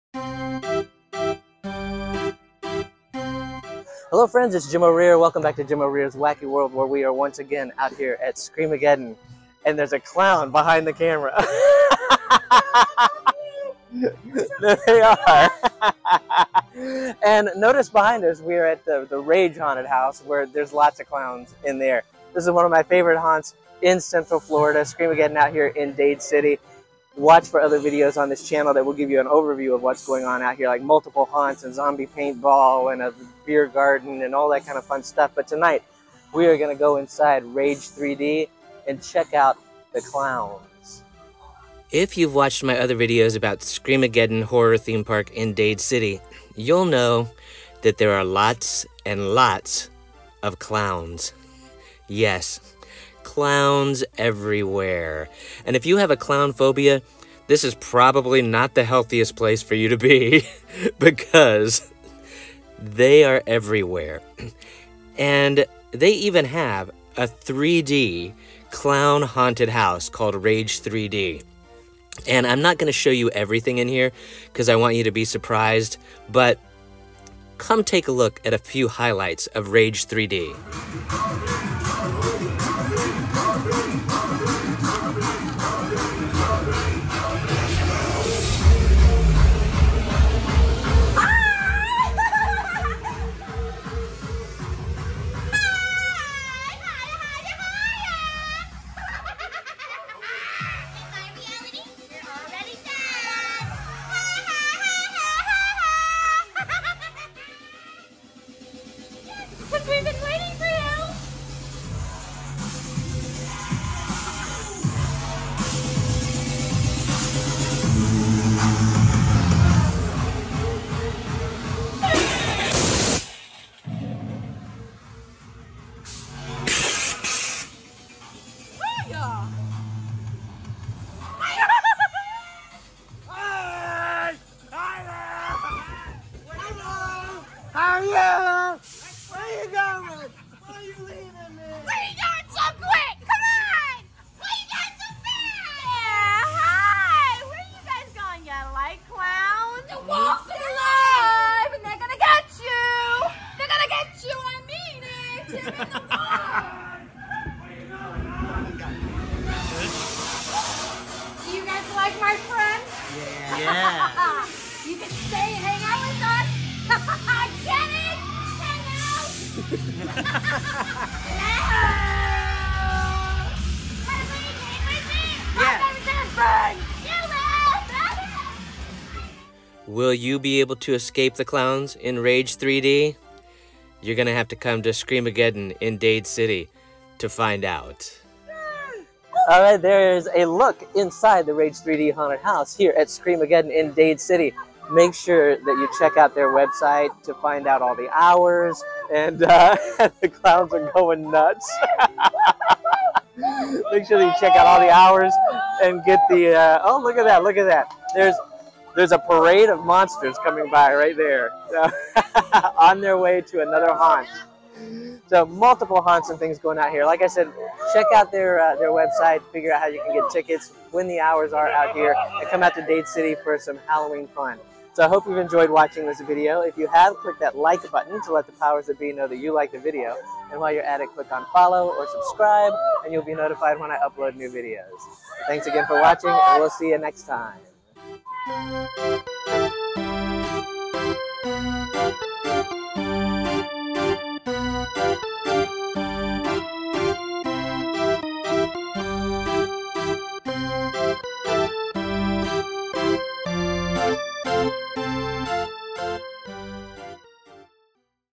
Escape The Clowns Inside The Scream-A-Geddon Rage 3D Haunted House! Join Our Walkthrough!
Prepare for a wild ride through Rage 3D at Scream-A-Geddon! In this haunted house walkthrough, you'll face off against sinister clowns in a mind-bending 3D environment. Can you escape the madness, or will you be trapped in their twisted funhouse forever? Join us for a pulse-pounding adventure full of terror!

CHAPTERS:
00:00 Introduction
01:24 Rage 3d Walkthrough
03:08 Recap